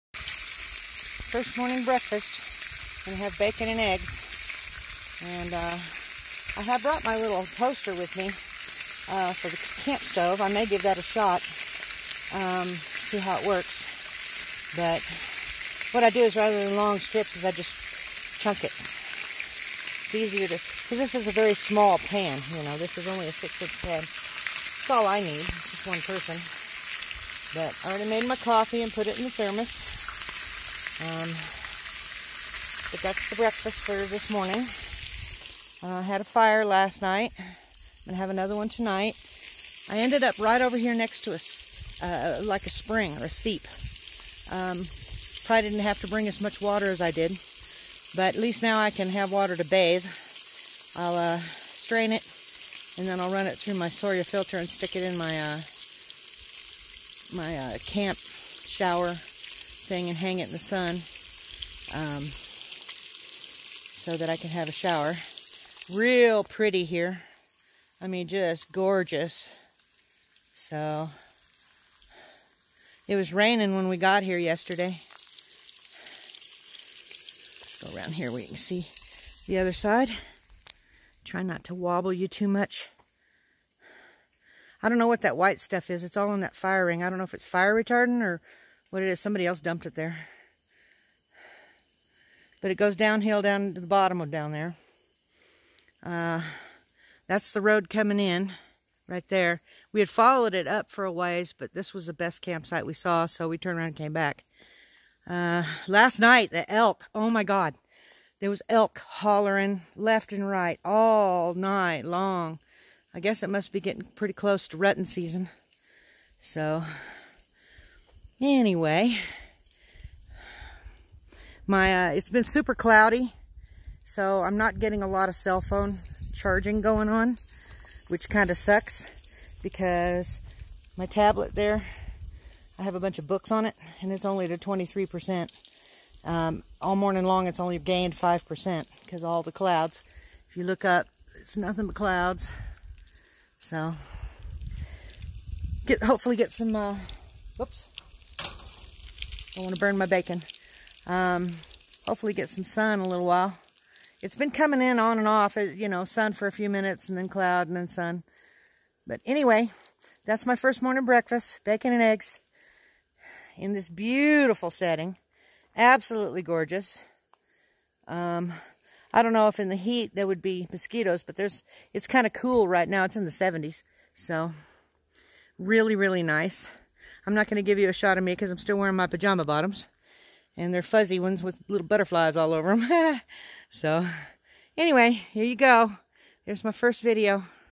Breakfast first morning Lincoln national forest
Showing the campsite and breakfast